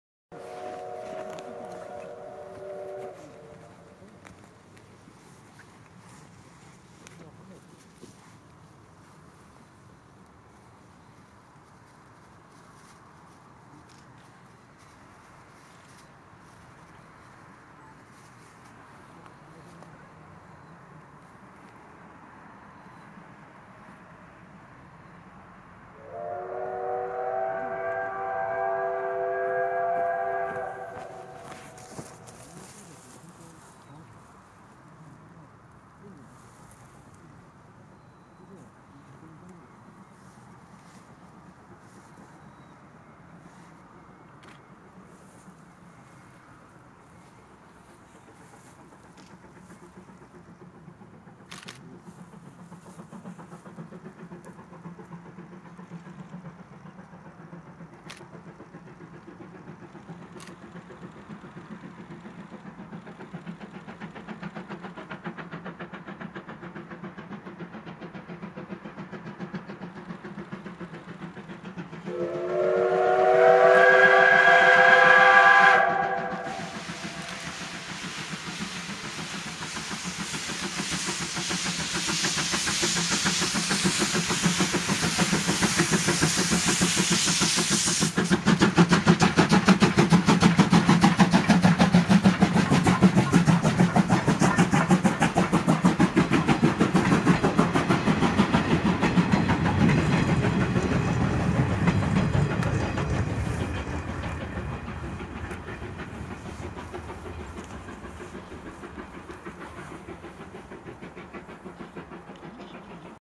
SLもおか 雪が残る天矢場を登る
2018　1月真岡鉄道の茂木-天矢場間の有料撮影地からです。ここから狙うと撮影地手前で機関士がドレインを切ってくれるので豪快な写真が撮れることが多いです。今回も迫力ある煙とドレインで良いのが撮れました！